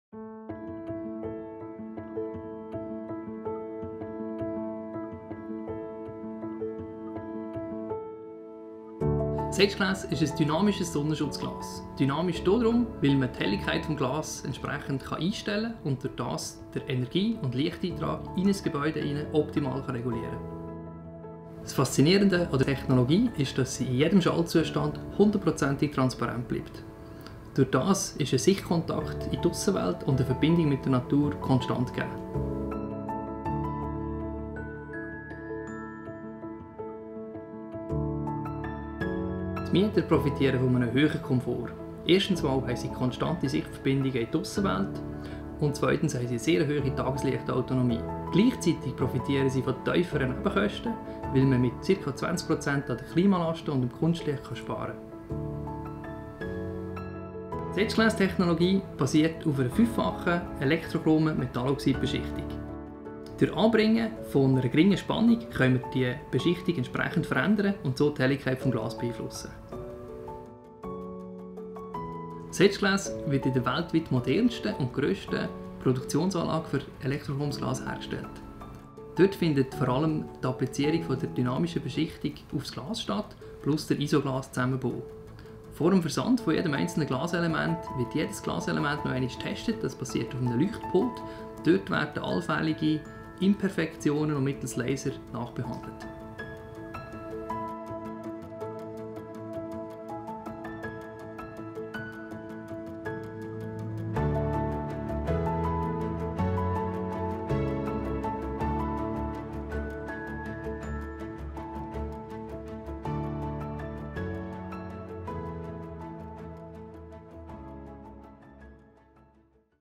Alpha Building | SageGlass
When planning the innovative seven-story ALPHA Building, Stephan Häusler, the owner and architect cleverly incorporated innovative and intelligent solutions. These included SageGlass Harmony® smart windows, which tint and clear according to the weather conditions and occupants’ needs to provide solar and glare control.